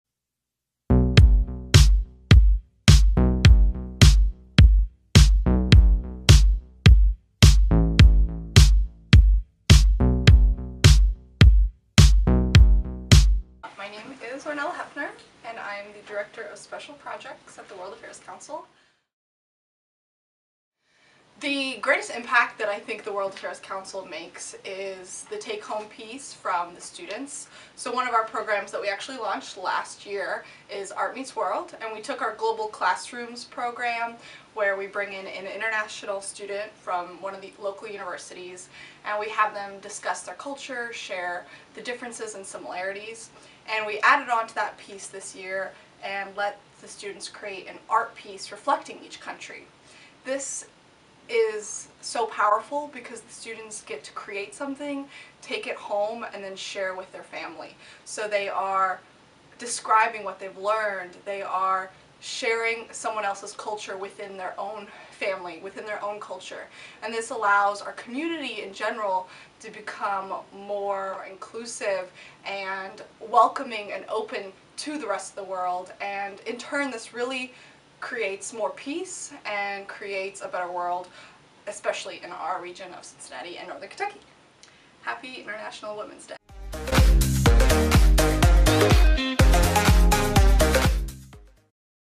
Arnela Hepner, Director of Special Projects
In honor of International Women's Day, we highlighted the "Women of WAC."

Arnela and her family immigrated from Bosnia when she was a just little girl. Her roots and culture are ingrained in her work every day as she provides a new vision of our global organization.

As the Director of Special Projects, Arnela focuses on the development of membership, program support through grant writing, and special large events such as the gala and annual education events.